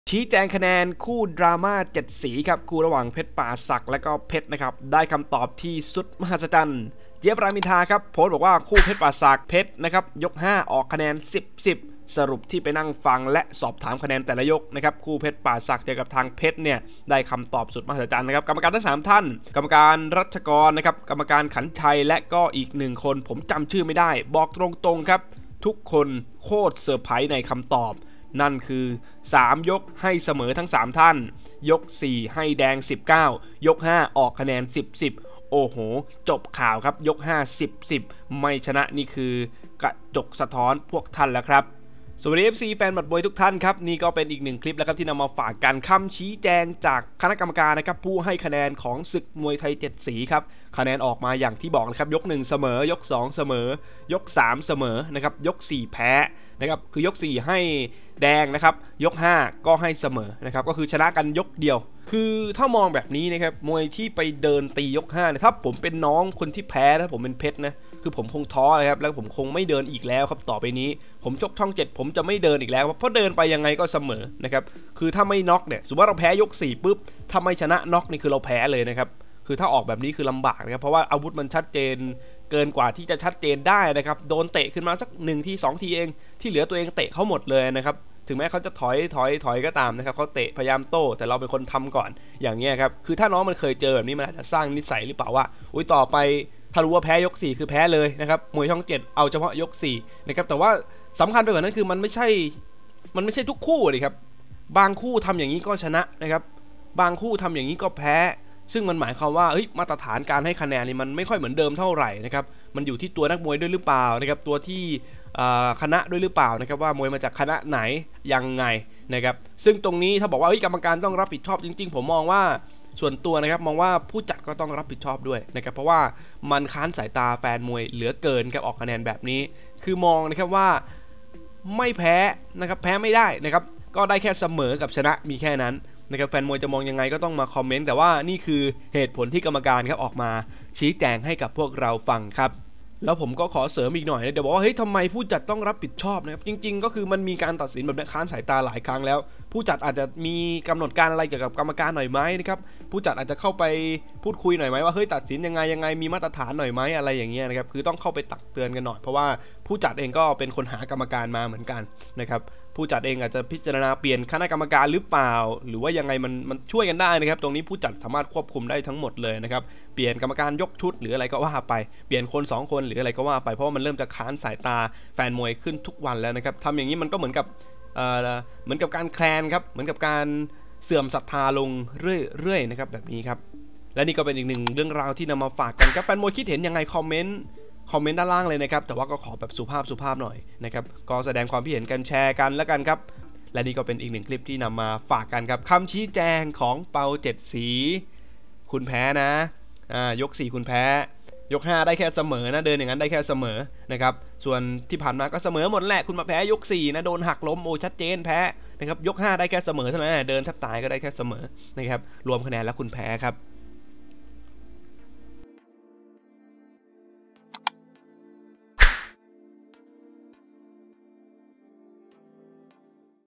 ชี้แจงคะแนน คู่ดราม่า 7 สี คู่ เพชรป่าศักดิ์ vs เพชร ได้คำตอบที่สุด มหัศจรรย์ ์
โพสจาก พี่เจี๋ยบ รามอินทรา
สรุป ที่ไปนั่ง ฟัง และสอบถาม คะแนน แต่ละยก"
คู่เพชรป่าศักดิ์ vs เพชร "ได้คำตอบ"ที่สุด "มหัศจรรย์"
"กรรมการ "ทั้ง3ท่าน" อ.รัชกร "อ.ขันชัย"และอีกคน"ผมจำชื่อ"ไม่ได้" บอกตรงๆทุกคน"โคตรเซอไพร"ในคำตอบ"
"นั่นคือ "3ยก ให้เสมอ"ทั้ง3ท่าน" 4ให้แดง 10-9 ✅
ยก5 ออก คะแนน 10-10 โอ้โห้! จบข่าว ยก5 10-10ไม่ชนะนี่"คือ "กระจกสะท้อน"พวกท่านแล้วครับ